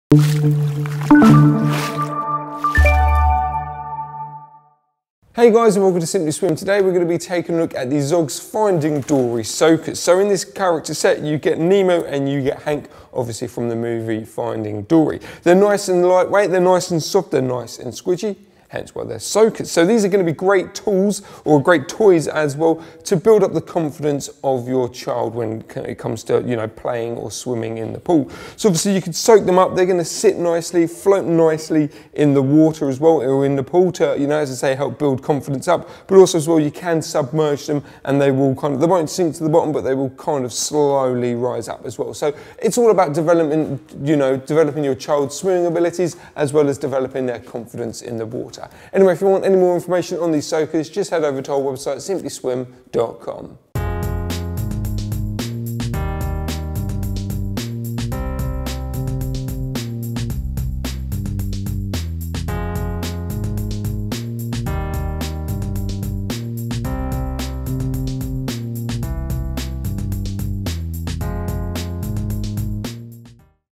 Zoggs Nemo and Hank Soakers
Have fun with the kids using the Zoggs Nemo and Hank soakers which feature the grouchy Hank and Nemo. These fun toys will help to give little ones more confidence in and around water.